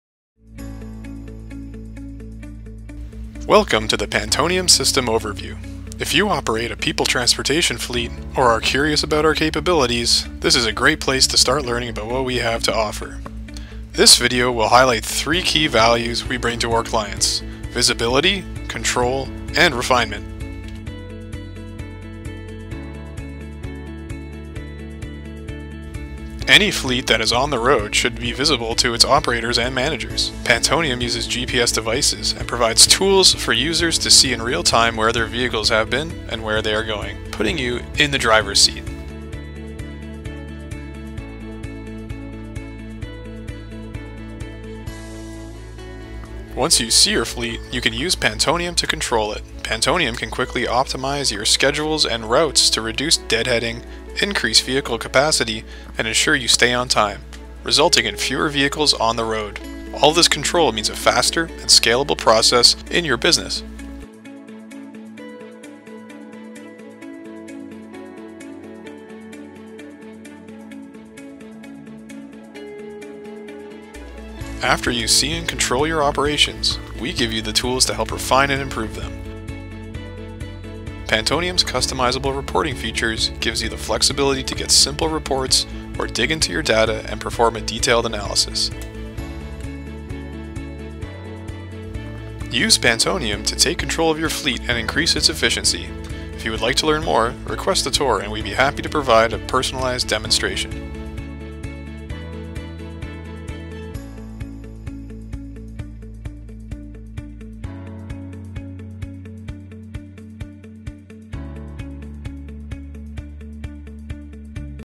Pantonium System Overview
This video gives a brief introduction and overview to the Pantonium system.